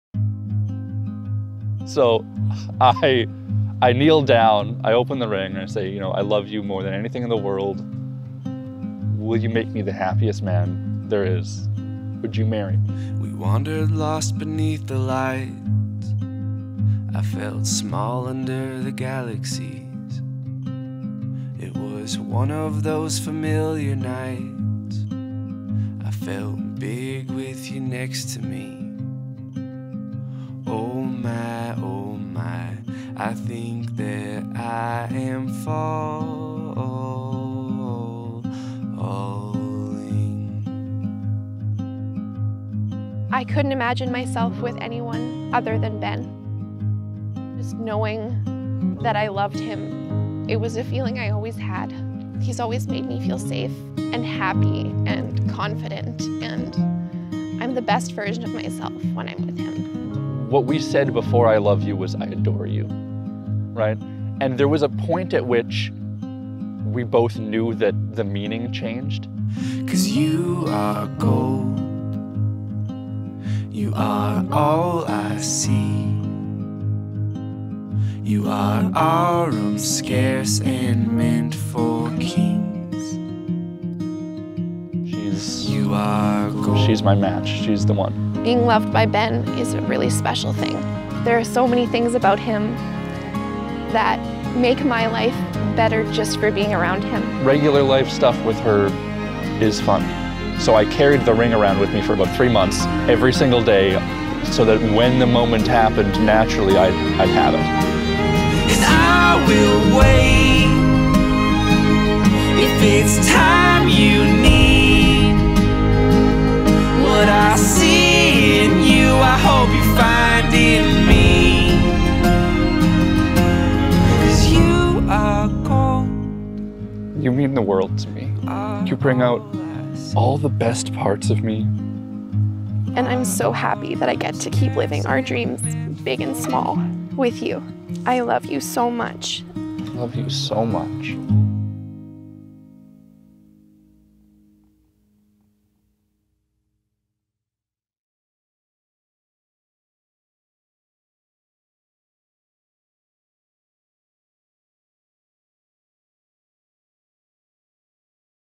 Abbey & Ben Engagement Video
Abbey and Ben are tying the knot in just a few short days! Congratulations to the soon-to-be-newlyweds, and happy engagement!

Music ID: MB01YOLMXA6GFW4